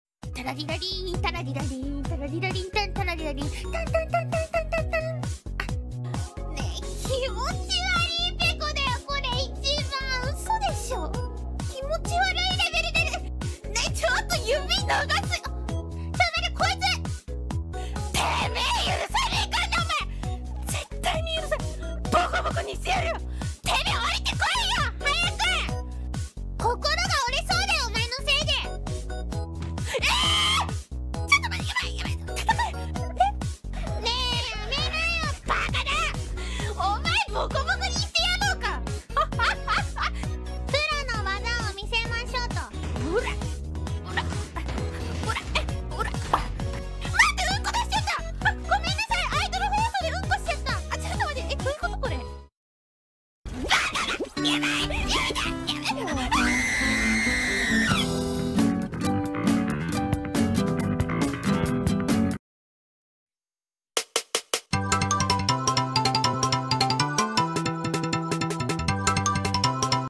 Random pekora rabbit 6 | Animal hololive
Pekora rabbit random video part 6
Pekora rabbit getting bullied so many time and become a food for hungry crocodile

Sorry for miss translate i don't speak japanese, i just copy the subtitle from clip i used for this content

This channel already monetized you don't have to donate me anymore, but if you want to donate me i appreciate it